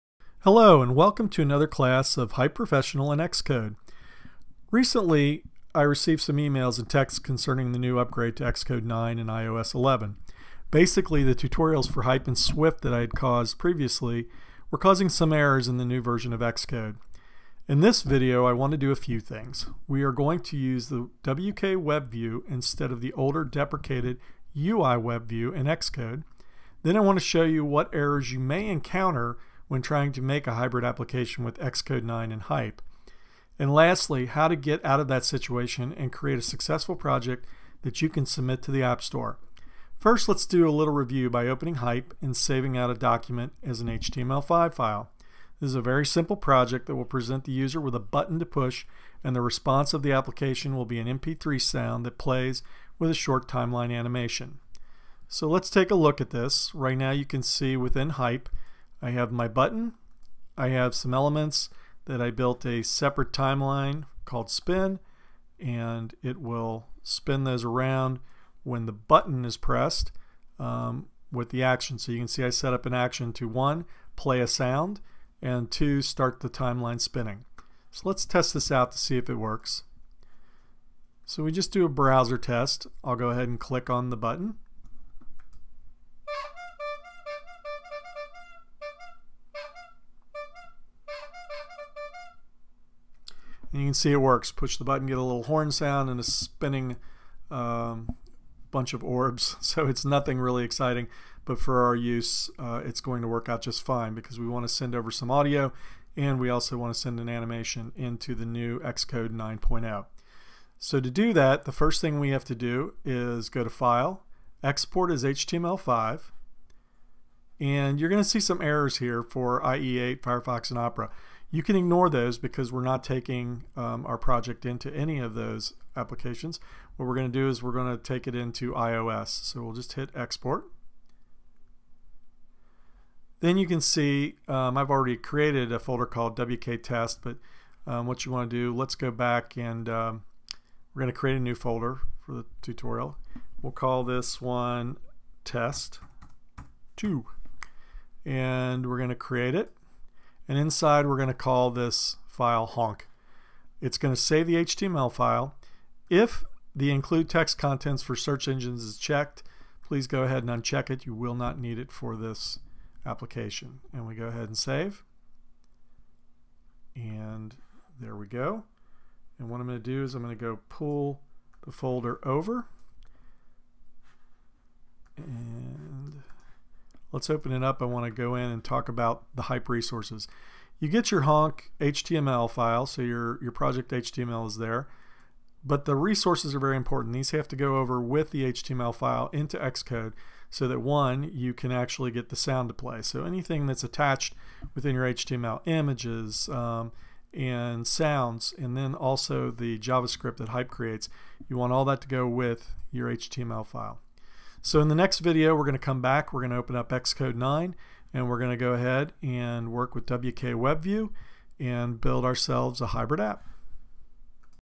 Making an Xcode 9.0 application with Hype
In this video we will use the WKWebView object to embed Hype content into our Xcode 9.0 project. using Swift 4.0 with a target of iOS11.

This is the first part in a two part video tutorial on how to do this.